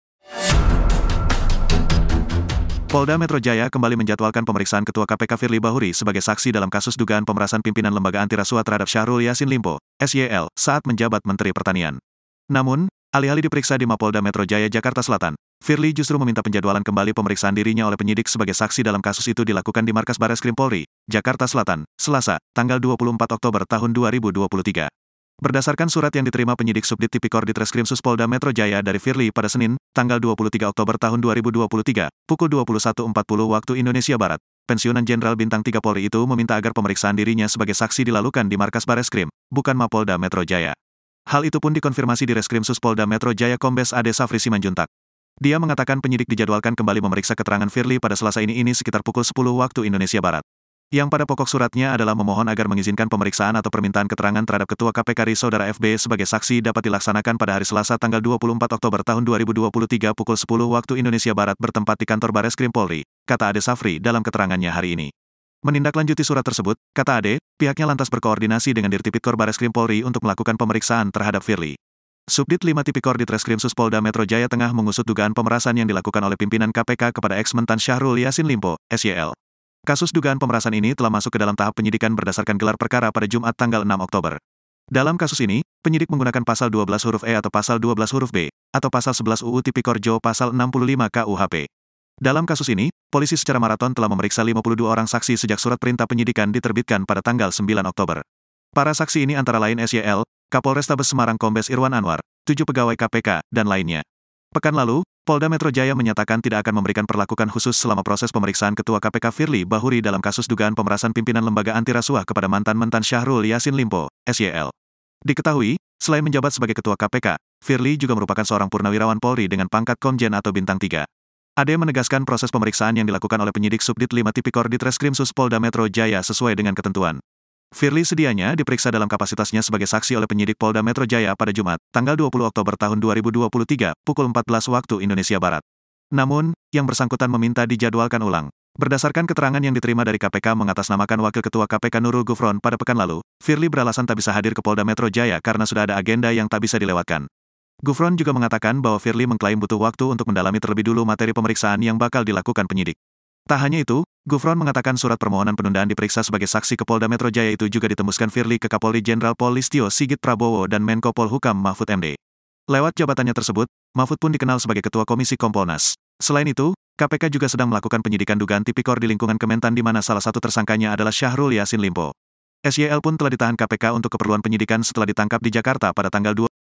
KASUS Dugaan Pemeresan SYL, Ketua KPK Firli Bahuri Malah Minta Diperiksa di Bareskrim Polri
Pemeriksaan dugaan adanya pemerasan yang dilakukan Ketua Komisi Pemberantasan Korupsi (KPK) Firli Bahuri dijadwalkan hari ini, Selasa (24/10/2023). Namun, Firli disebut enggan diperiksa di Markas Kepolisian Daerah (Mapolda) Metro Jaya. Firli bakal diperiksa sebagai saksi dalam dugaan pemerasan terhadap eks Menteri Pertanian Syahrul Yasin Limpo.